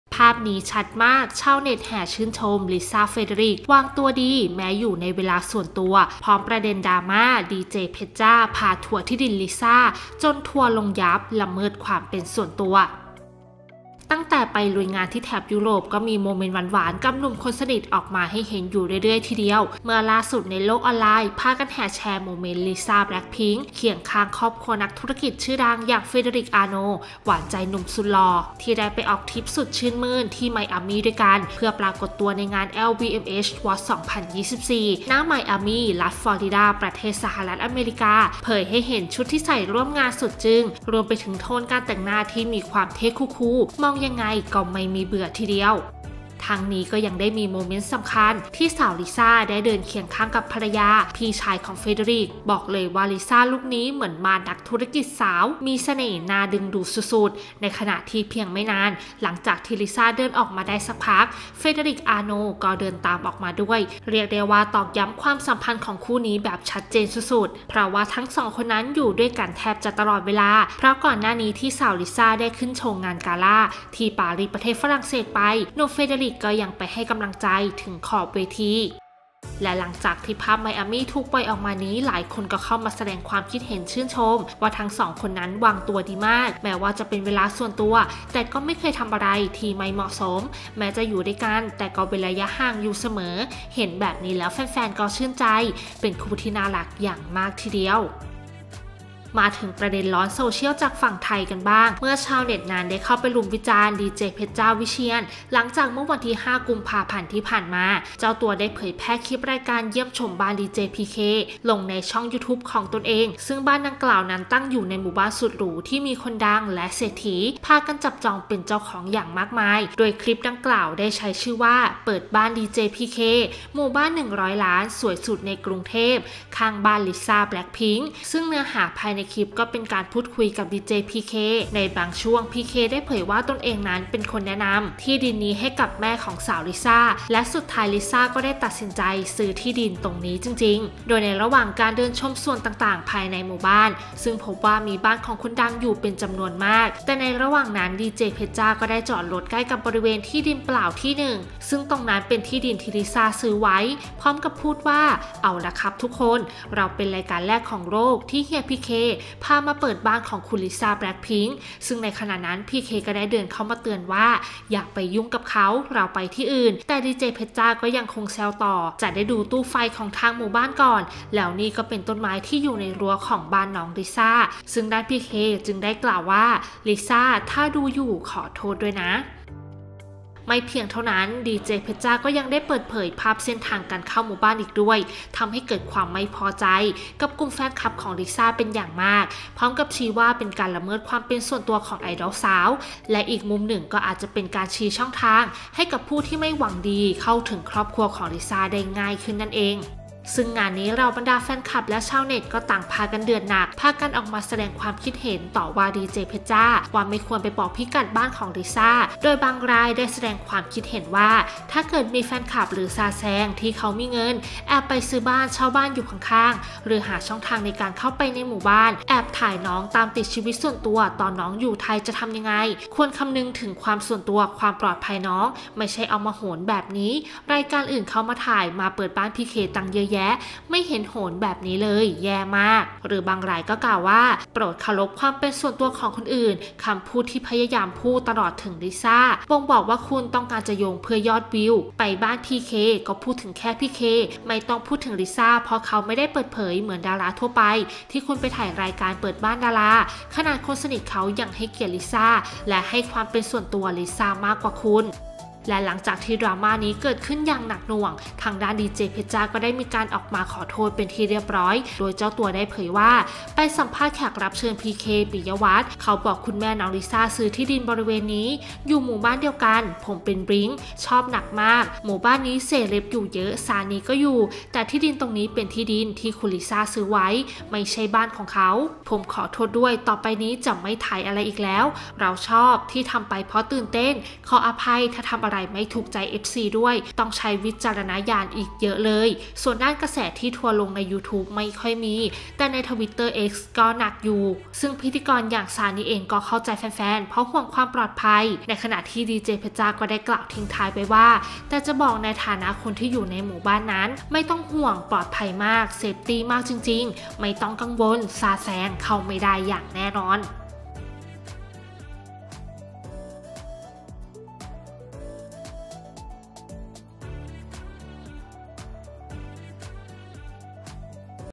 ชาวเน็ตแห่ชื่นชม ลิซ่า เฟรเดอริก วางตัวดี พร้อมประเด็นดราม่า เพชรจ้า พาทัวร์ที่ดินลิซ่า
ตั้งแต่ไปลุยงานที่แถบยุโรป ก็มีโมเมนต์หวานๆกับหนุ่มคนสนิทออกมาให้เห็นเรื่อยๆเลยทีเดียว เมื่อล่าสุด แฟนๆในโลกออนไลน์พากันแห่แชร์โมเมนต์ ลิซ่า BLACKPINK เคียงข้างครอบครัวของ เฟรเดอริก อาโนลด์ หวานใจหนุ่มหล่อ ที่ได้ไปออกทริปสุดชื่นมื่นที่ไมอามี่ เพื่อปรากฏตัวในงาน LVMH watches 2024 ที่ไมอามี่ รัฐฟลอริด้า ประเทศสหรัฐอเมริกา เผยให้เห็นชุดที่ใส่ร่วมงาน รวมไปถึงโทนการแต่งหน้าที่มีความเท่คูลดูยังไงปังมากๆ